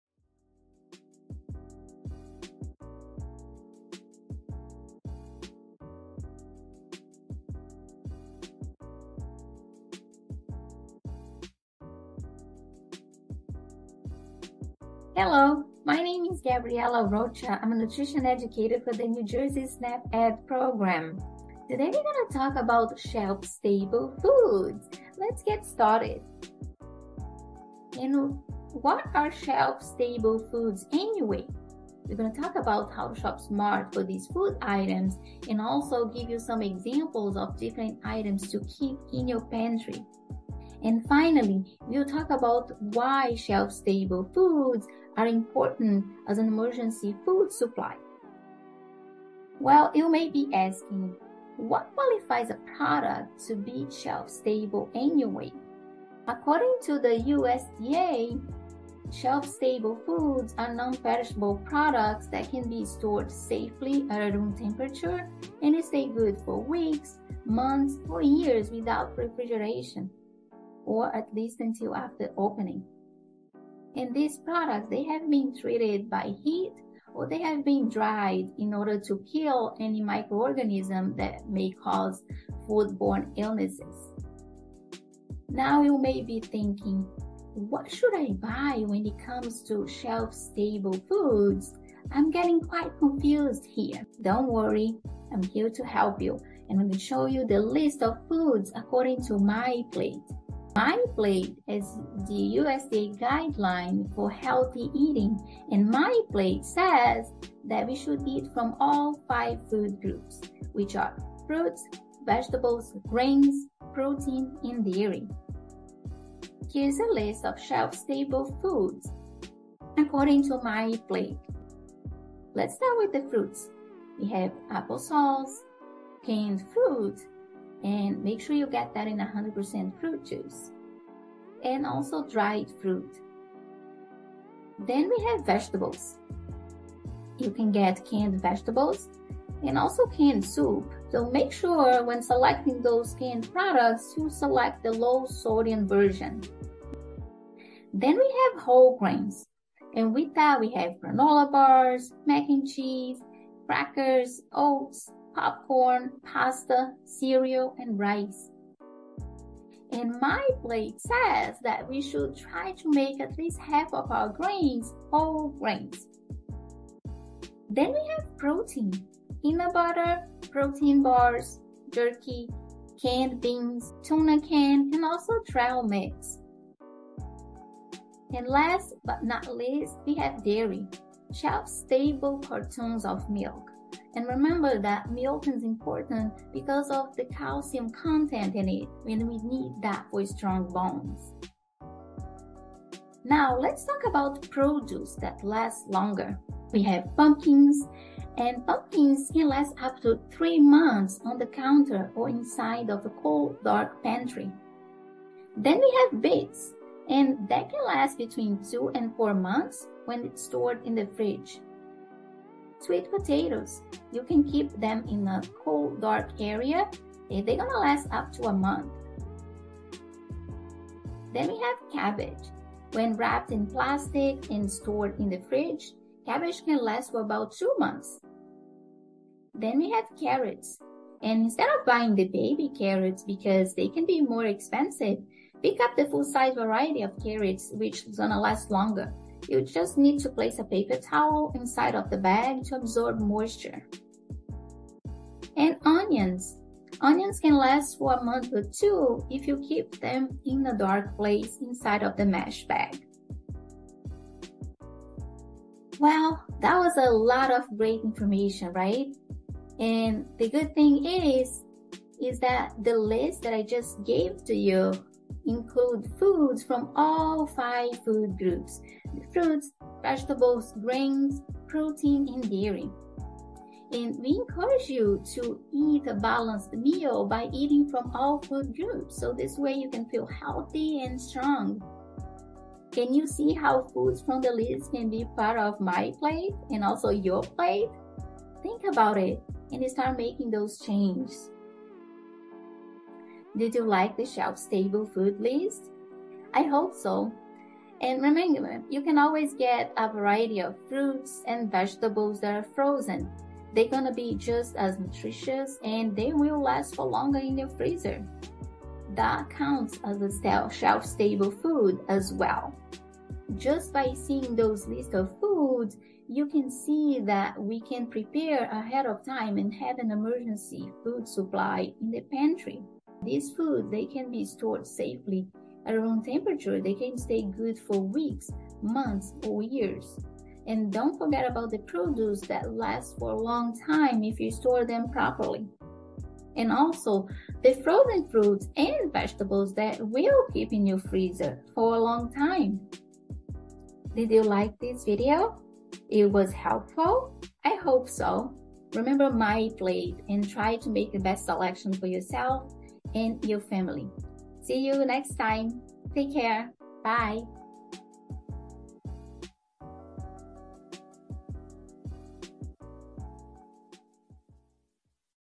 NJ SNAP-Ed WellnessWednesday: Shelf-Stable Foods
In this video, you will learn about what shelf-stable foods are, how to shop smart for them, and how to store them safely. You'll also get to see which shelf-stable foods fit into the MyPlate nutrition guidelines!